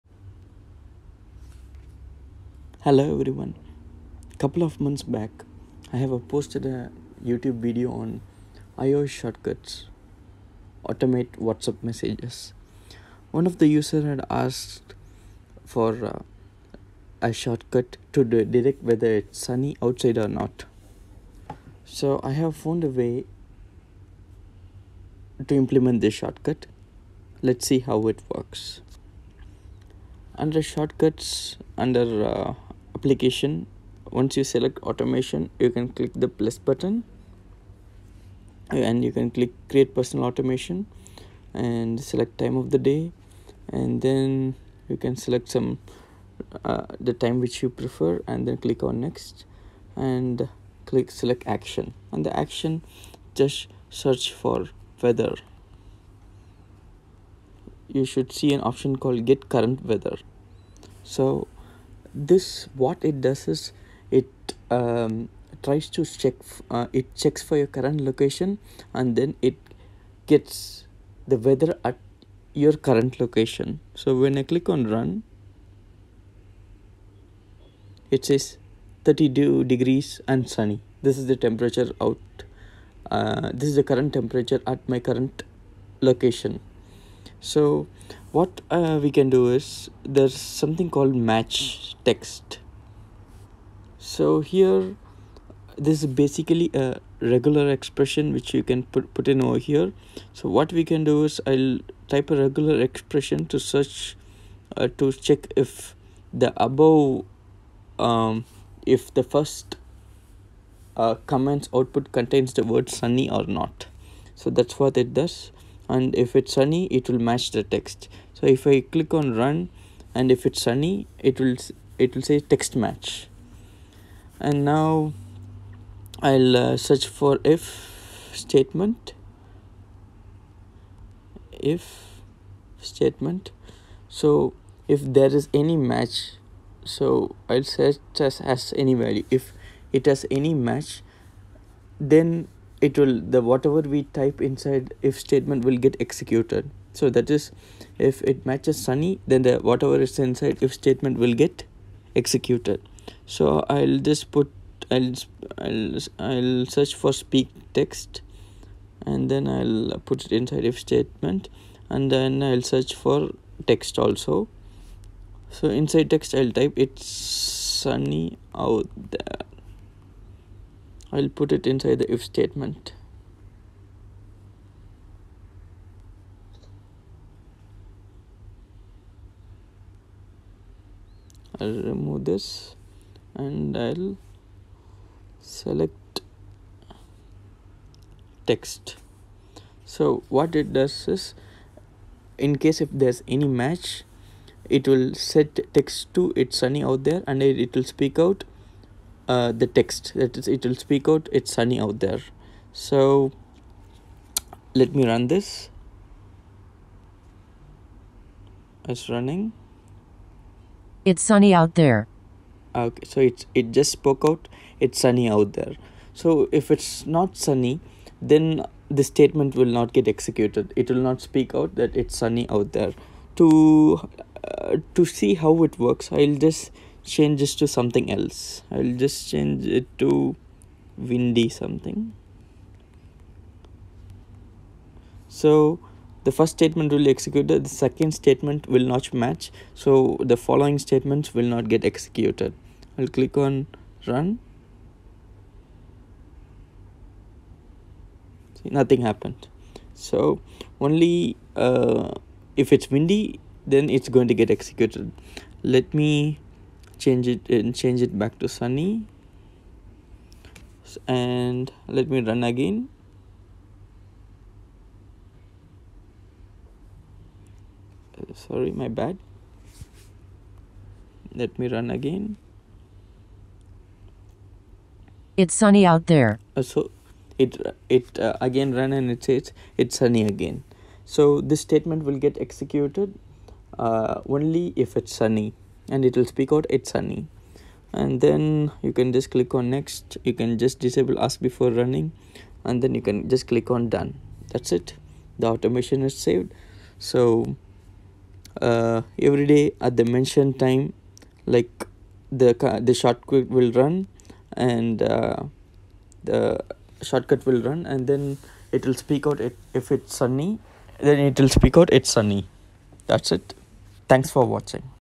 iOS Shortcuts: Notify if weather is “Sunny”
In this video we will see how to use iOS shortcut to check if it's sunny out there at current location and prompt a message saying "It's sunny"